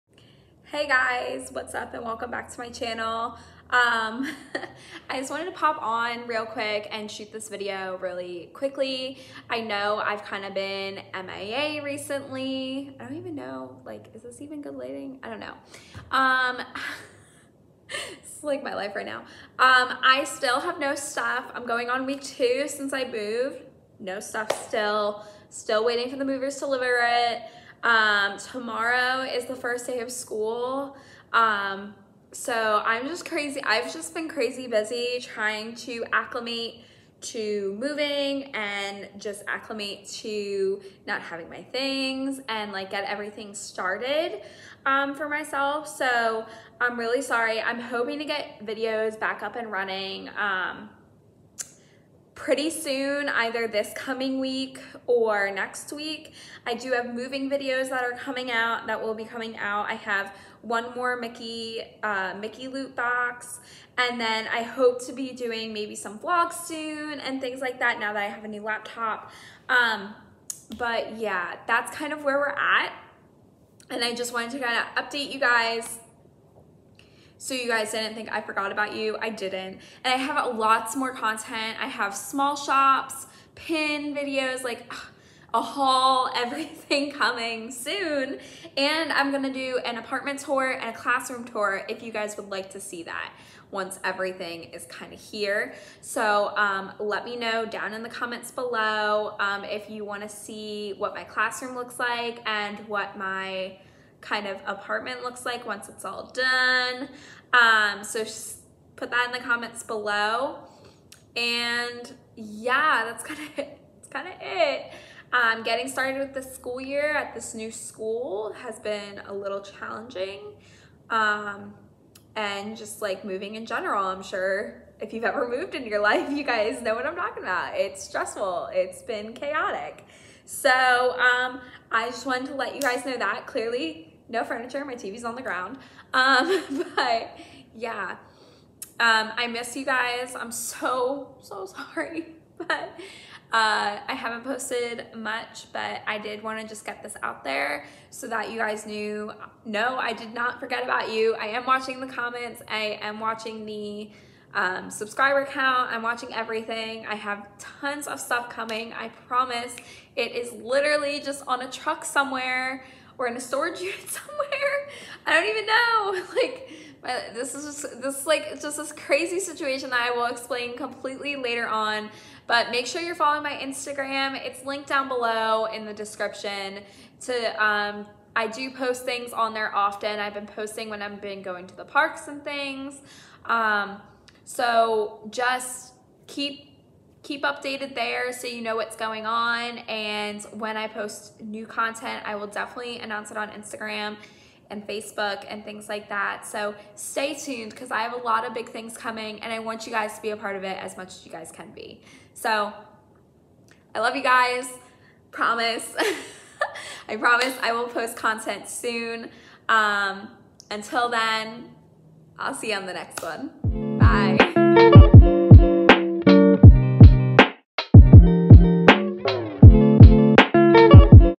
Life and Channel Update! | No I did not forget about you all!
I want to thank everyone who has supported me up until this point! I promise new content is coming soon and I am working on it! Here is just a quick little update of everything going on!